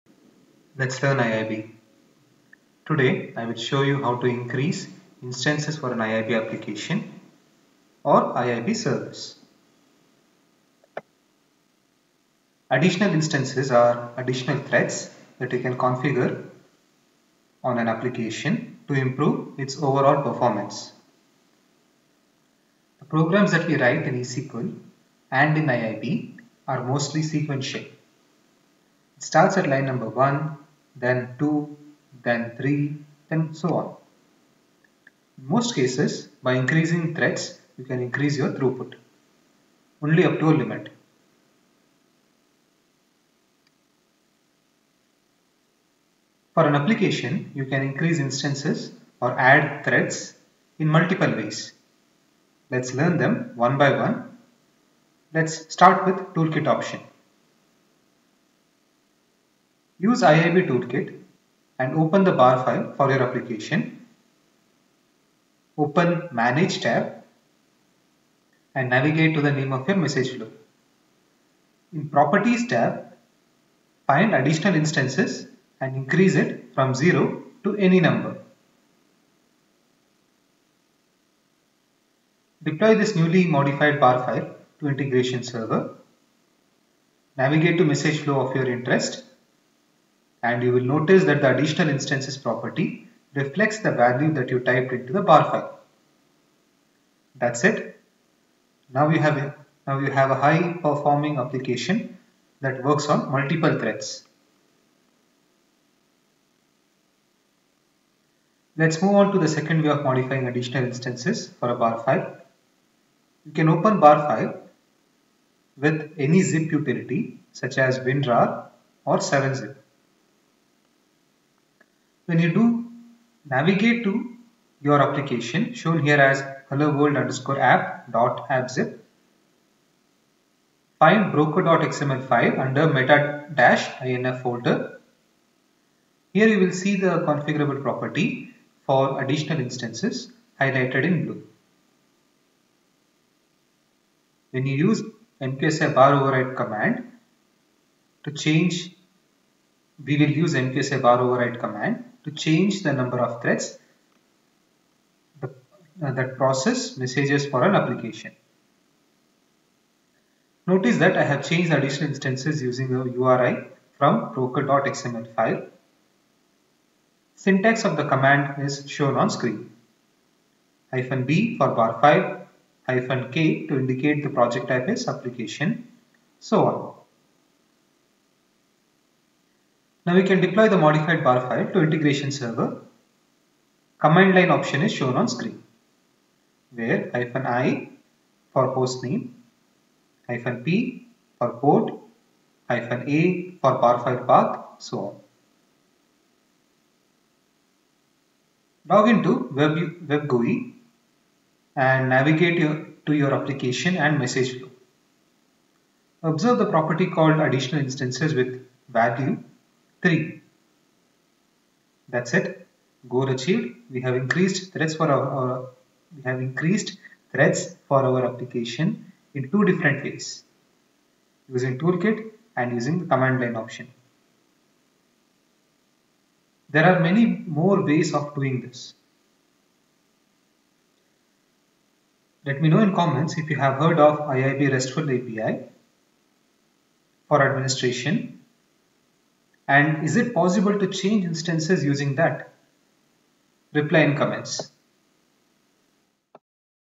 iib - how to increase additional instances - IBM Integration Bus
Let's learn iib
ibm integration bus, ibm integration bus training, ibm integration bus tutorials, websphere message broker

Today i will show you how to increase instances for an iib application or service.